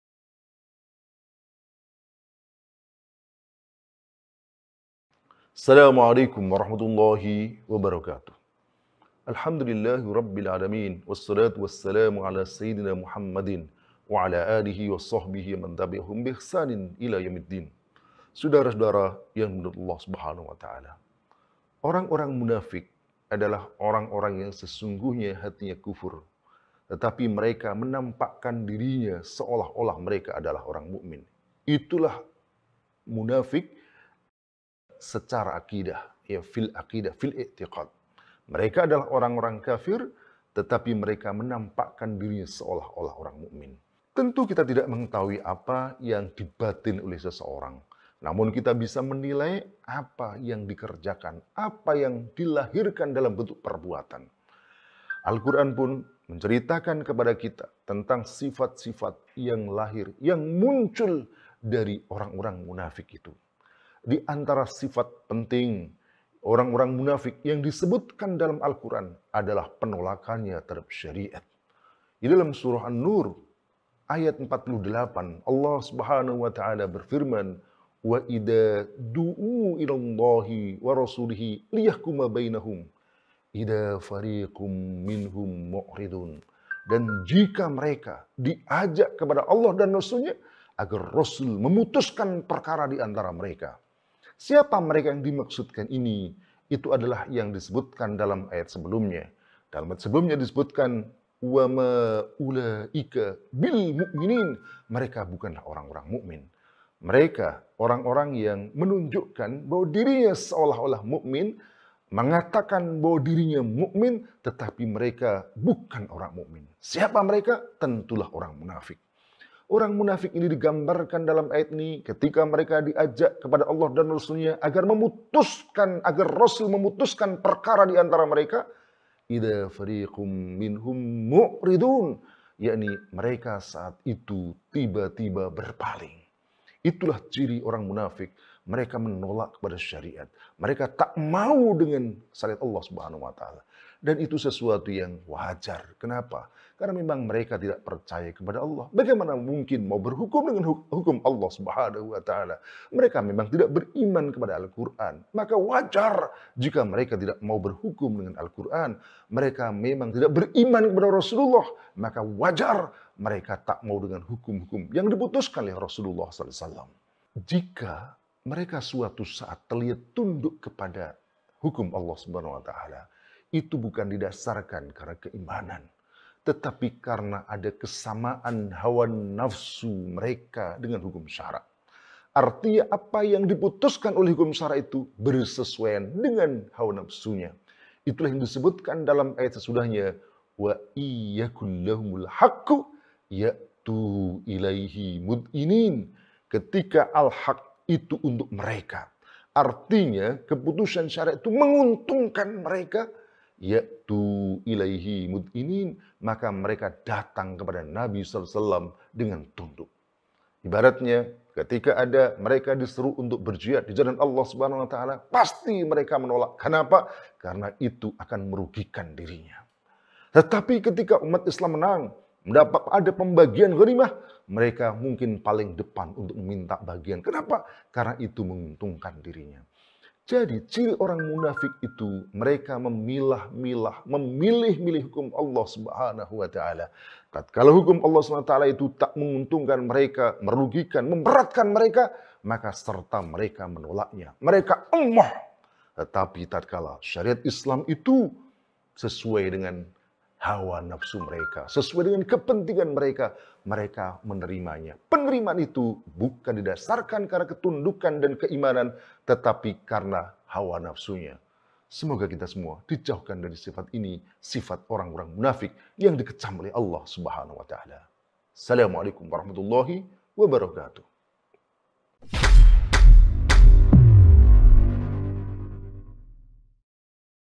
Orang-orang munafik | KH Rokhmat S Labib
Orang-orang munafik adalah orang-orang yang sesungguhnya hatinya kufur tetapi mereka menampakan dirinya seolah-olah mereka adalah orang mukmin itulah munafik secara akidah. Akidah mereka adalah orang-orang kafir tetapi mereka menampakan dirinya seolah-olah beriman, kita tidak mengetahui apa yang seseorang namun kita bisa menilai apa yang dikerjakan apa yang dilahirkan dalam bentuk perbuatan Alquran pun menceritakan kepada kita tentang sifat-sifat yang lahir yang muncul dari orang-orang munafik itu.  diantara Sifat penting orang-orang munafik yang disebutkan dalam Alquran adalah penolakan yang terjadi di dalam surah an-nur ayat 48
وَإِذَا دُعُوٓا۟ إِلَى ٱللَّهِ وَرَسُولِهِۦ لِيَحْكُمَ بَيْنَهُمْ إِذَا فَرِيقٌ مِّنْهُم مُّعْرِضُونَ

Artinya: Dan apabila mereka dipanggil kepada Allah dan rasul-Nya, agar rasul menghukum (mengadili) di antara mereka, tiba-tiba sebagian dari mereka menolak untuk datang.